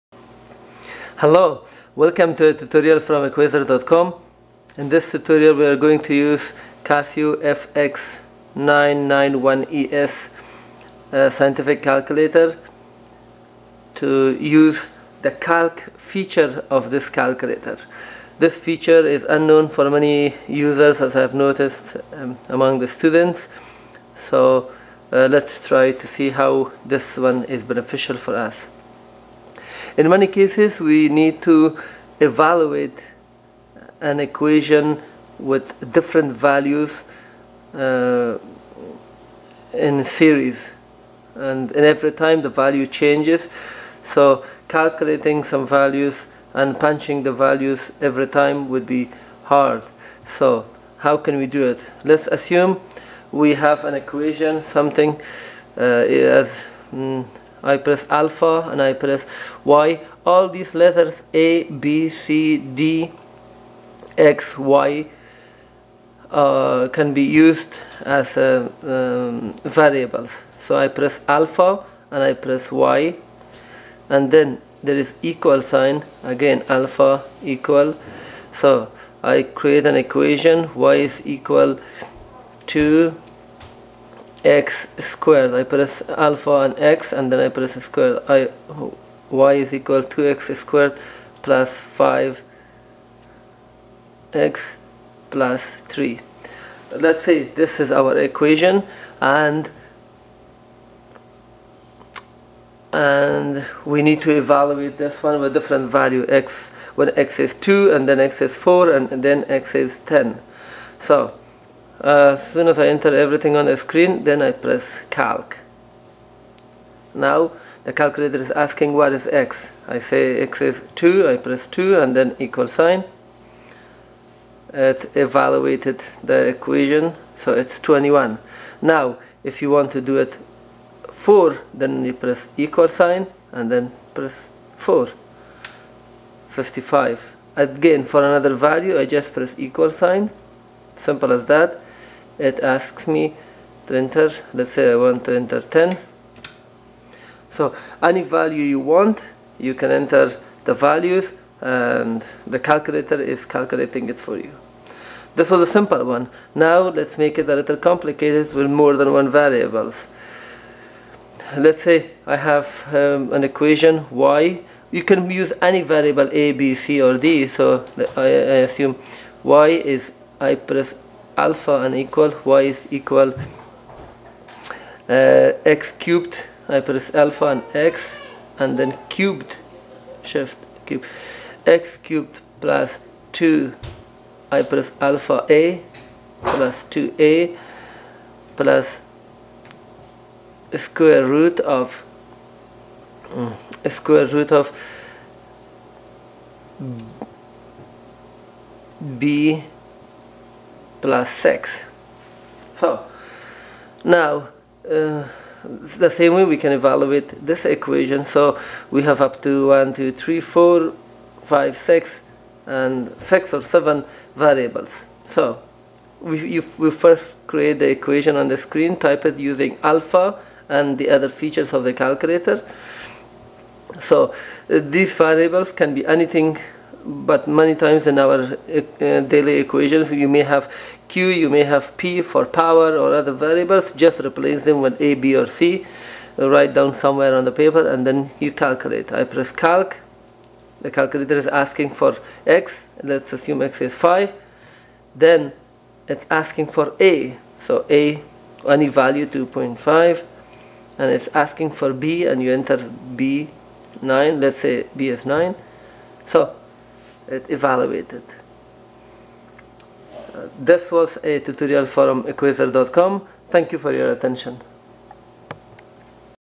Using CALC in Casio FX-115ES Plus, FX-991ES, FX-991ES Plus C
In this tutorial, you are going to learn how to use the CALC feature of the Casio FX-115ES Plus, Casio FX-991ES Plus scientific calculator provided by Equaser.com

Purchase Casio FX-991ES Plus calculator from Amazon

Please like 👍 the video and type simple word in the comment. It helps me a lot.